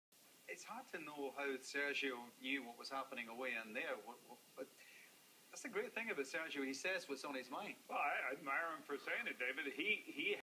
Random beer clink at Tpc Sawgrass
Two guys have an unusual beer on one knee in the background of Sky Sports coverage of The Players Championship in Sawgrass